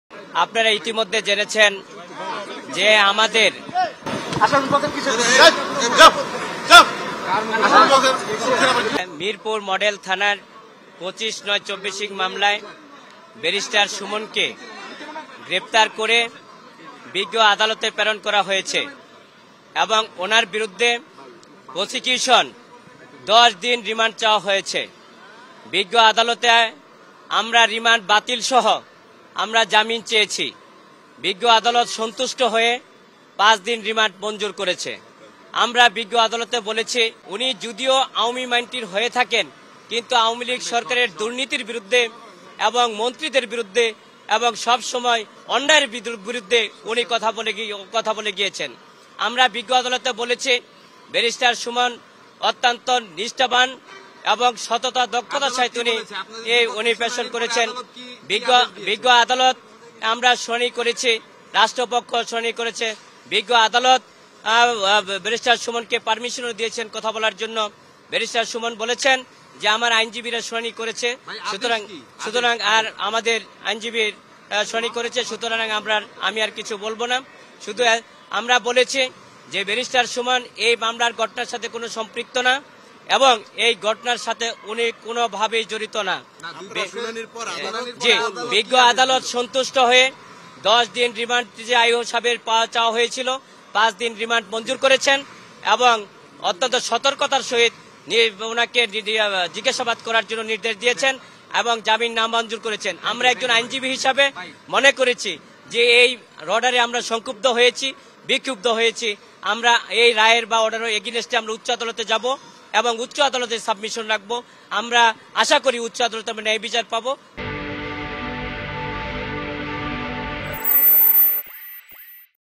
আসামি পক্ষের ব্রিফিং কিসের ? ব্যারিস্টার সুমনের আইনজীবীর মাথায় আঘাত | Barrister Suman | RSTA News
আদালতে ব্যারিস্টার সুমনের আইনজীবীর মাথায় আঘাত...